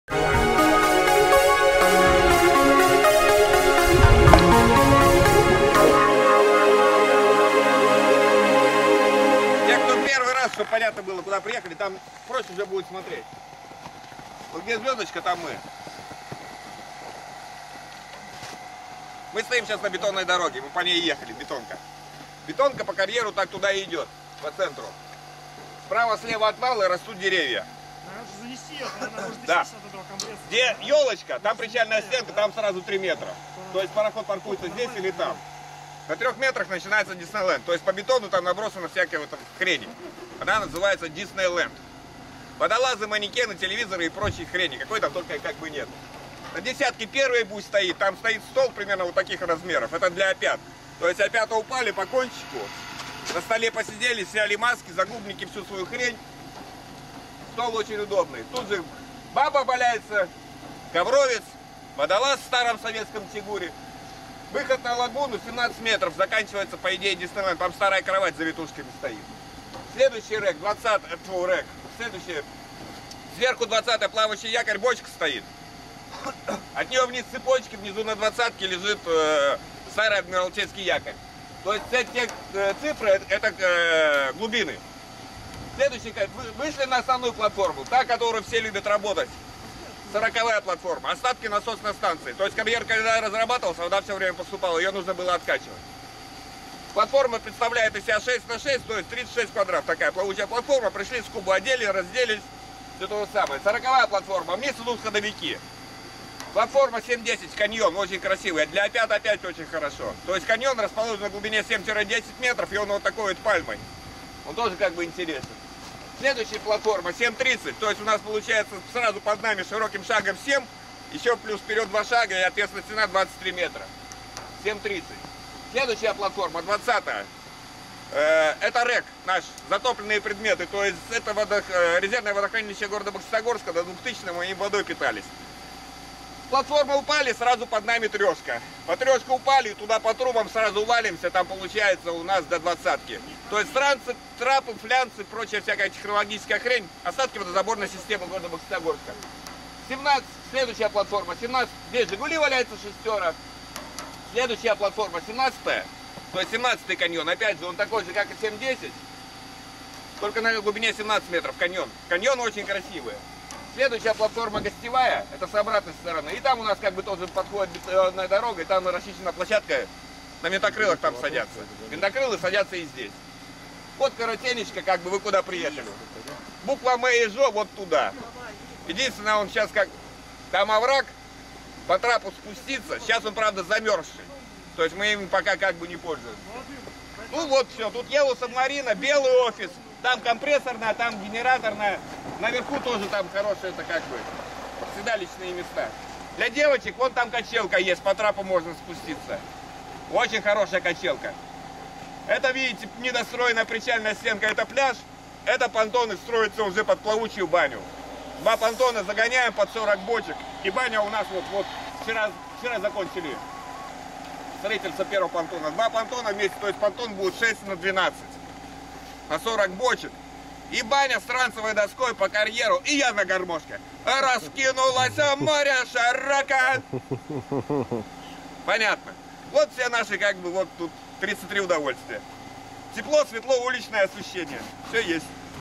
Брифинг Паши или Все 33 удовольствия :)
Это брифинг Паши с Бокситогорского карьера номер 17. Дайвинг клуб ОК.